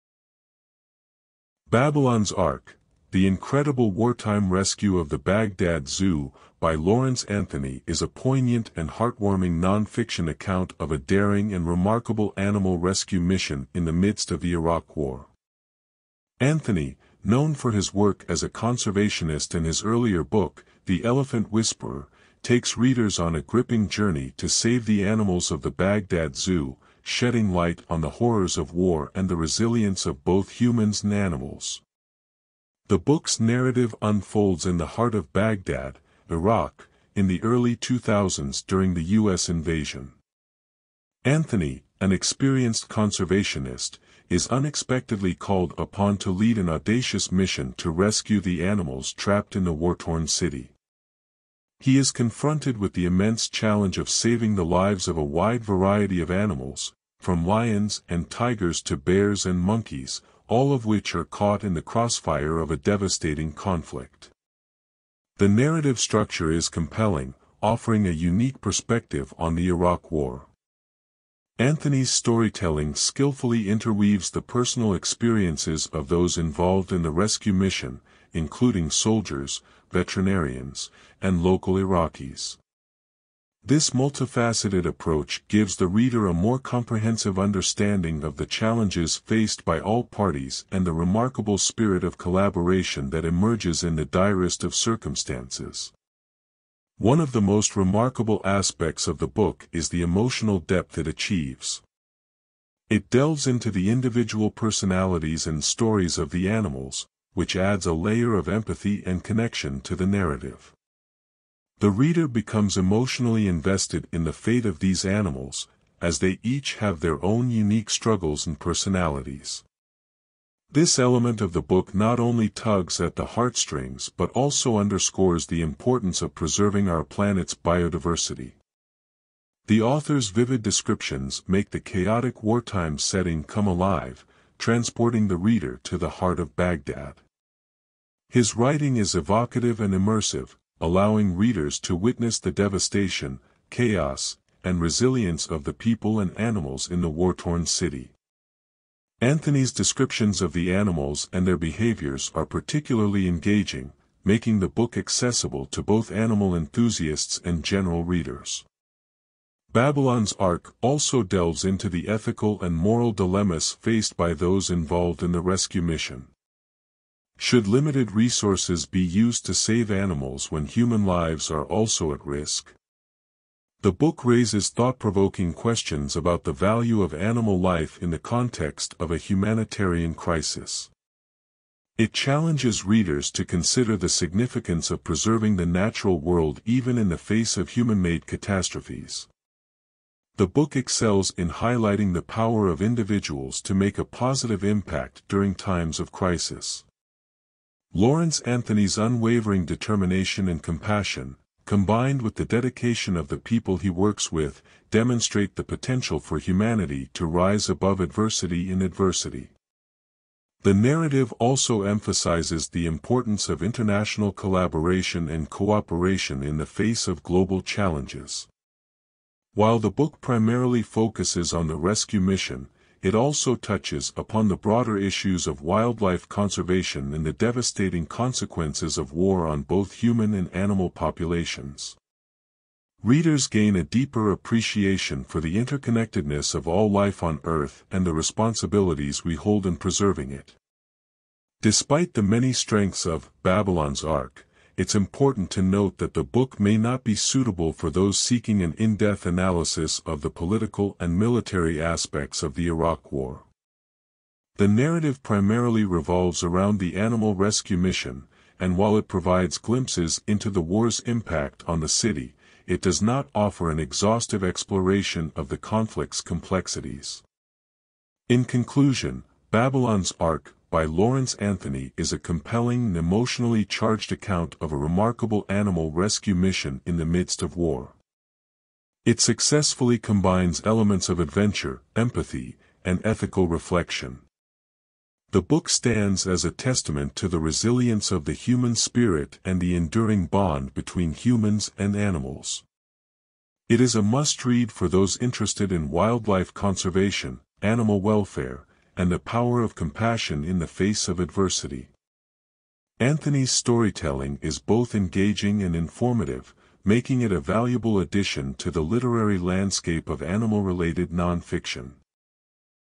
"Babylon's Ark" By Lawrence Anthony
"Babylon's Ark: The Incredible Wartime Rescue of the Baghdad Zoo" by Lawrence Anthony is a poignant and heartwarming non-fiction account of a daring and remarkable animal rescue mission in the midst of the Iraq war. Anthony, known for his work as a conservationist and his earlier book "The Elephant Whisperer," takes readers on a gripping journey to save the animals of the Baghdad Zoo, shedding light on the horrors of war and the resilience of both humans and animals.The book's narrative unfolds in the heart of Baghdad, Iraq, in the early 2000s during the U.S. invasion. Anthony, an experienced conservationist, is unexpectedly called upon to lead an audacious mission to rescue the animals trapped in the war-torn city. He is confronted with the immense challenge of saving the lives of a wide variety of animals, from lions and tigers to bears and monkeys, all of which are caught in the crossfire of a devastating conflict.The narrative structure is compelling, offering a unique perspective on the Iraq war. Anthony's storytelling skillfully interweaves the personal experiences of those involved in the rescue mission, including soldiers, veterinarians, and local Iraqis. This multifaceted approach gives the reader a more comprehensive understanding of the challenges faced by all parties and the remarkable spirit of collaboration that emerges in the direst of circumstances.One of the most remarkable aspects of the book is the emotional depth it achieves. It delves into the individual personalities and stories of the animals, which adds a layer of empathy and connection to the narrative. The reader becomes emotionally invested in the fate of these animals, as they each have their own unique struggles and personalities. This element of the book not only tugs at the heartstrings but also underscores the importance of preserving our planet's biodiversity.The author's vivid descriptions make the chaotic wartime setting come alive, transporting the reader to the heart of Baghdad. His writing is evocative and immersive, allowing readers to witness the devastation, chaos, and resilience of the people and animals in the war-torn city. Anthony's descriptions of the animals and their behaviors are particularly engaging, making the book accessible to both animal enthusiasts and general readers."Babylon's Ark" also delves into the ethical and moral dilemmas faced by those involved in the rescue mission. Should limited resources be used to save animals when human lives are also at risk? The book raises thought-provoking questions about the value of animal life in the context of a humanitarian crisis. It challenges readers to consider the significance of preserving the natural world even in the face of human-made catastrophes.The book excels in highlighting the power of individuals to make a positive impact during times of crisis. Lawrence Anthony's unwavering determination and compassion, combined with the dedication of the people he works with, demonstrate the potential for humanity to rise above adversity and adversity. The narrative also emphasizes the importance of international collaboration and cooperation in the face of global challenges.While the book primarily focuses on the rescue mission, it also touches upon the broader issues of wildlife conservation and the devastating consequences of war on both human and animal populations. Readers gain a deeper appreciation for the interconnectedness of all life on Earth and the responsibilities we hold in preserving it.Despite the many strengths of "Babylon's Ark," it's important to note that the book may not be suitable for those seeking an in-depth analysis of the political and military aspects of the Iraq war. The narrative primarily revolves around the animal rescue mission, and while it provides glimpses into the war's impact on the city, it does not offer an exhaustive exploration of the conflict's complexities.In conclusion, "Babylon's Ark" by Lawrence Anthony is a compelling and emotionally charged account of a remarkable animal rescue mission in the midst of war. It successfully combines elements of adventure, empathy, and ethical reflection. The book stands as a testament to the resilience of the human spirit and the enduring bond between humans and animals. It is a must-read for those interested in wildlife conservation, animal welfare, and the power of compassion in the face of adversity. Anthony's storytelling is both engaging and informative, making it a valuable addition to the literary landscape of animal-related non-fiction.